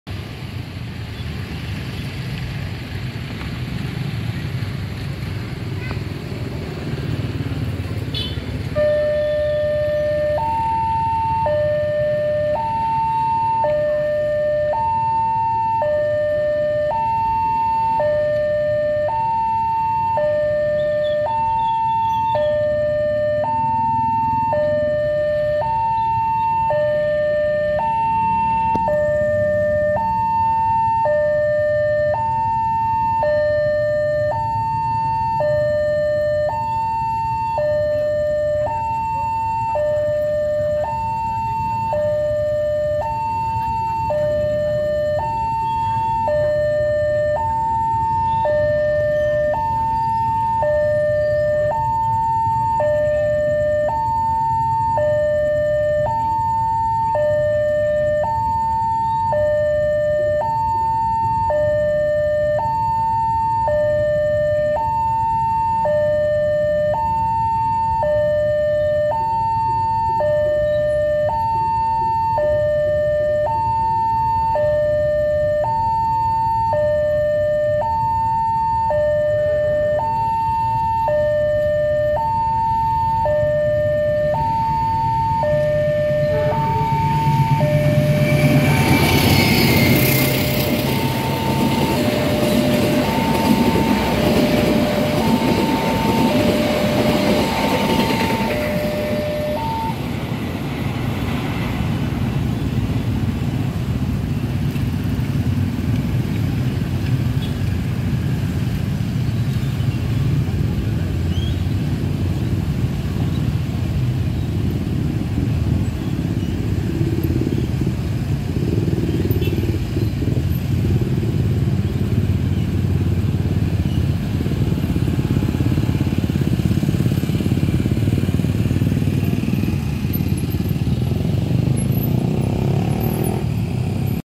Selamat Malam KA Wijayakusuma
Kereta Api Wijayakusuma Relasi: Cilacap - Ketapang (Banyuwangi) dengan Lokomotif CC206, melaju di JPL 349 Jl. Timoho Yogyakarta, 08 November 2021.

HUNTING KERETA API
INDONESIAN RAILWAYS
RAILROAD CROSSING